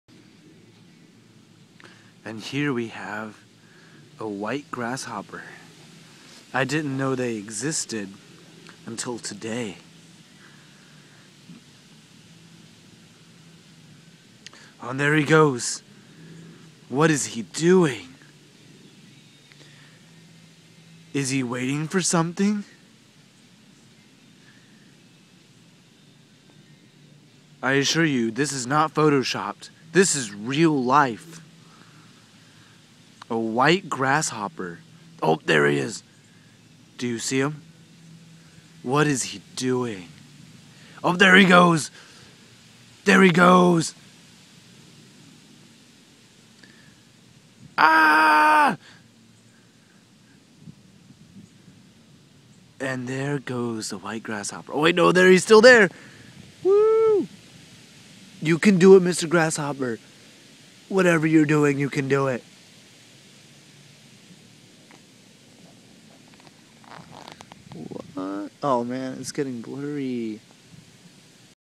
The Rare White Grasshopper pt.1
Watch as I film the rare white grasshopper.